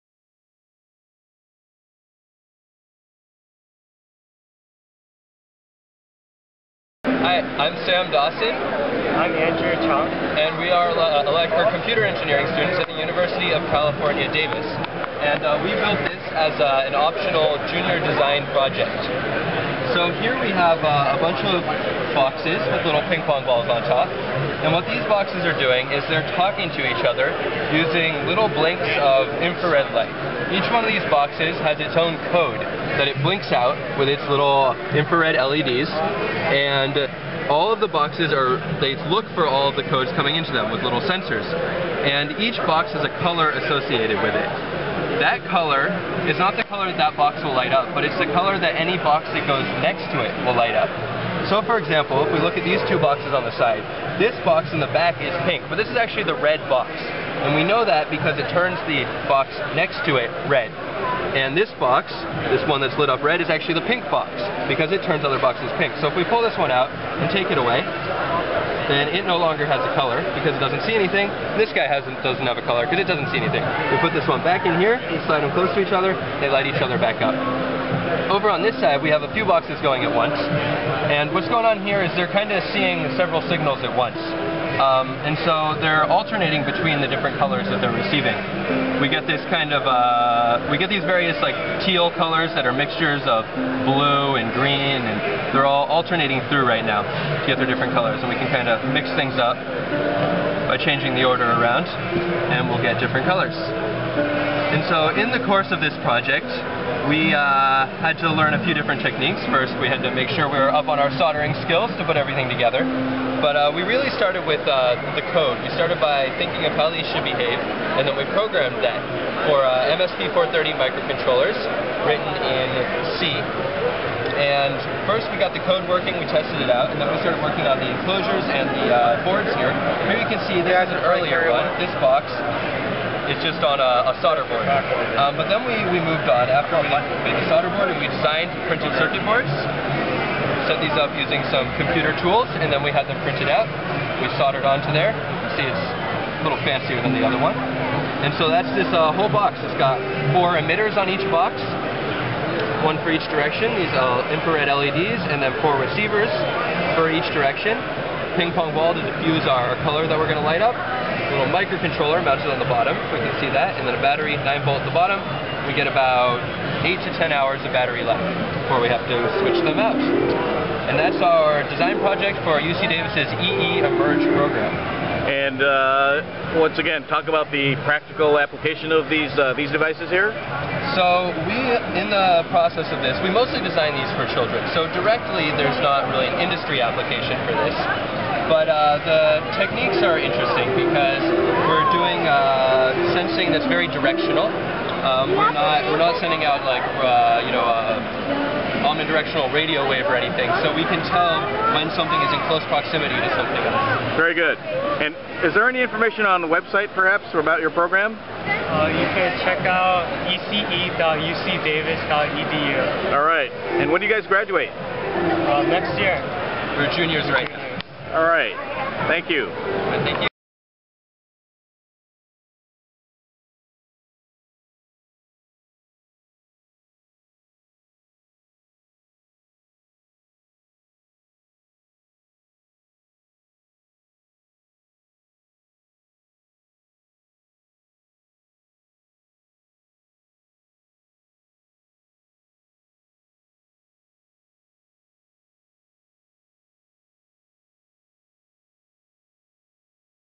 Intelligent Infrared Sensors at Bay Area Maker Faire 2014
University of California, Davis engineering students Sam Dawson and Andrew Chung demonstrate their intelligent sensor project at Maker Faire Bay Area.